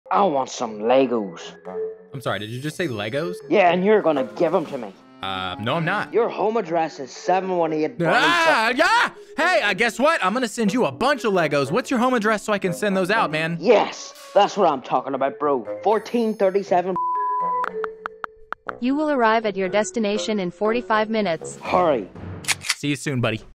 Scammer Kid.
This kid tries to extort me for free Legos. Time for some street justice.